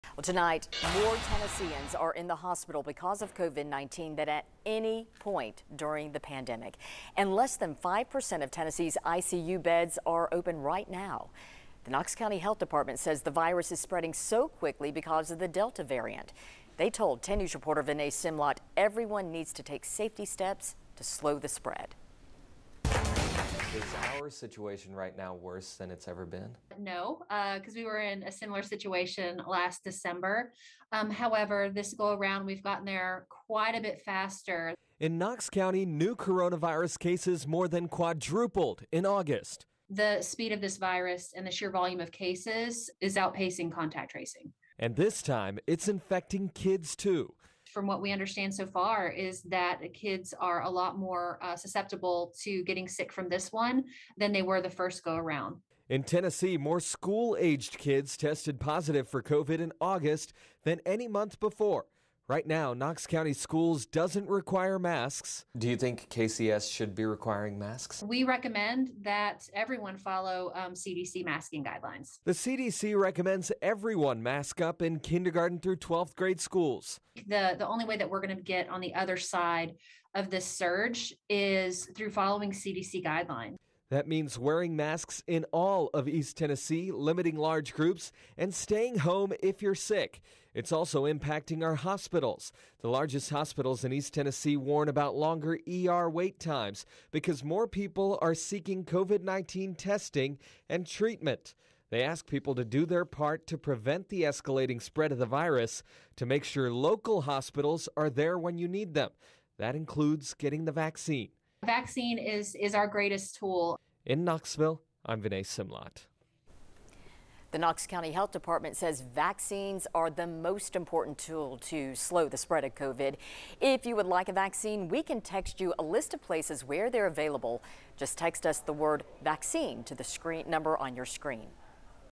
More Tennesseans are now in the hospital for COVID-19 than at any point in the pandemic
Less than five percent of Tennessee's ICU beds are open right now. The Knox County Health Department says new cases more than quadrupled in August.